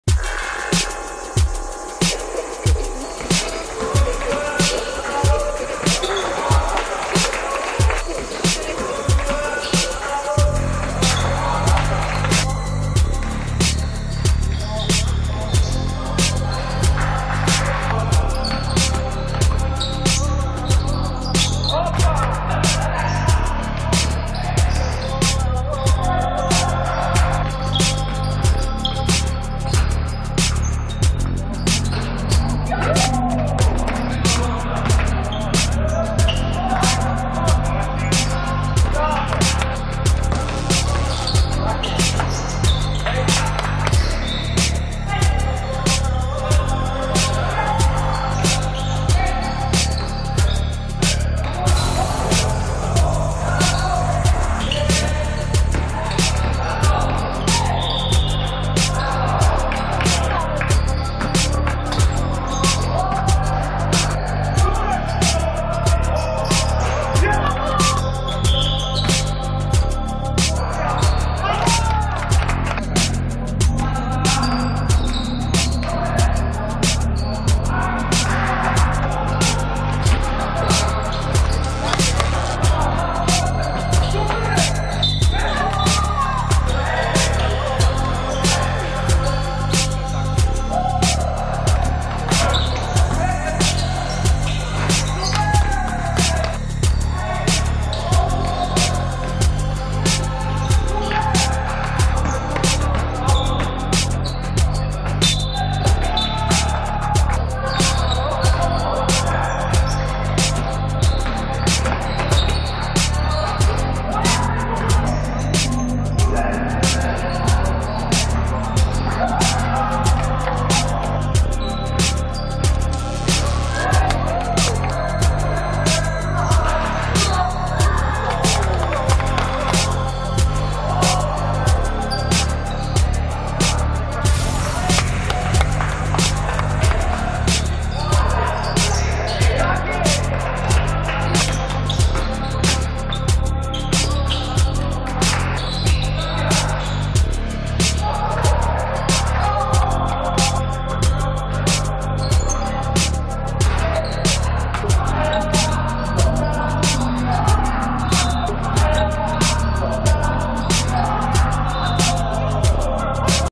16 04 Florida vs Kentucky – Liga Amerykańska 7
FLORIDA 90:51 KENTUCKY
J.Zakrzewski 25pkt 22zb 4stl 4blk
J.Żebrowski 15pt 24zb K.Koszałka 25pt